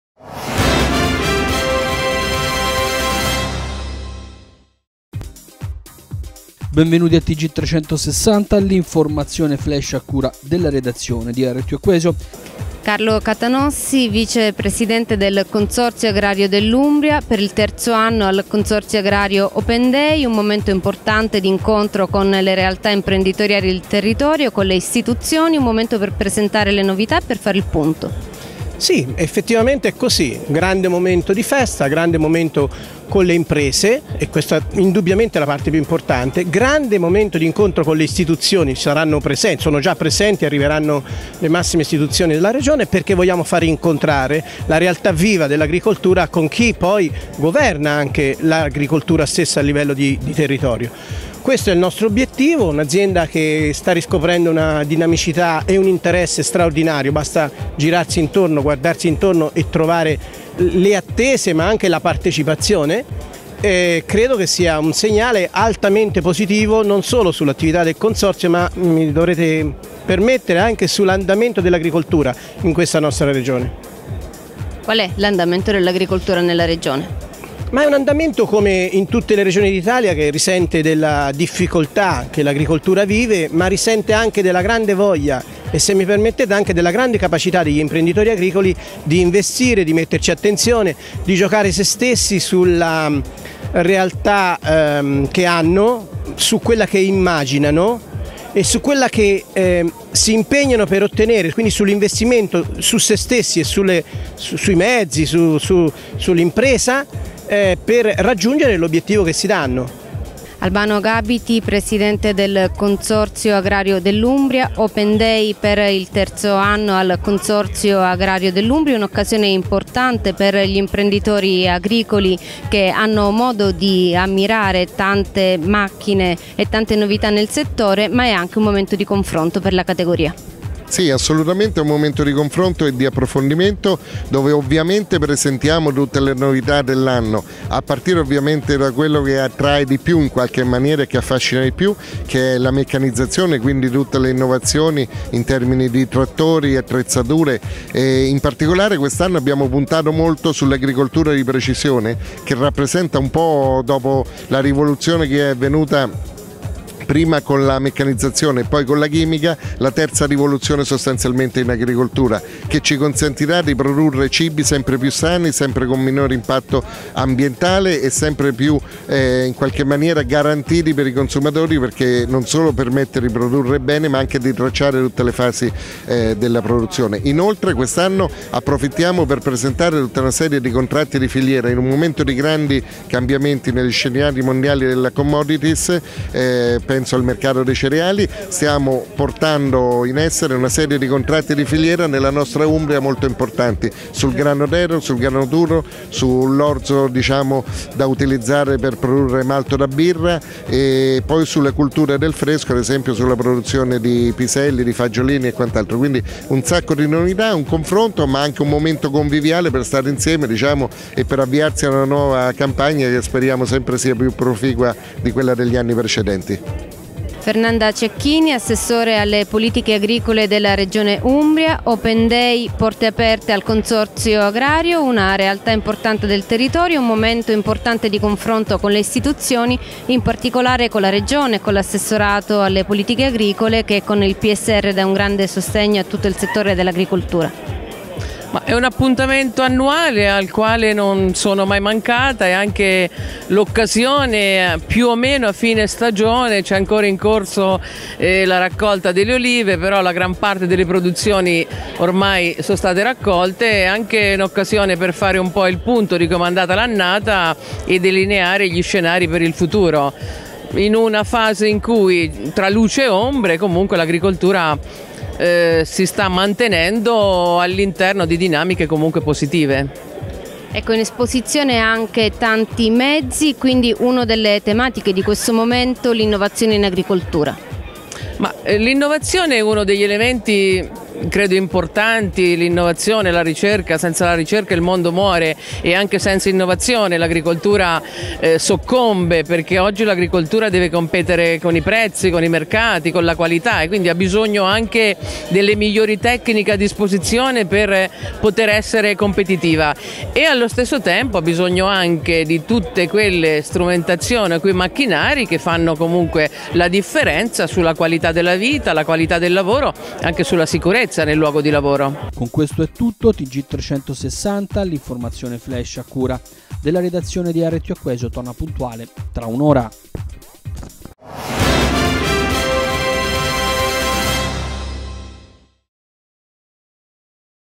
TG360 nr.1157
edizione del
speciale open day al consorzio agrario di perugia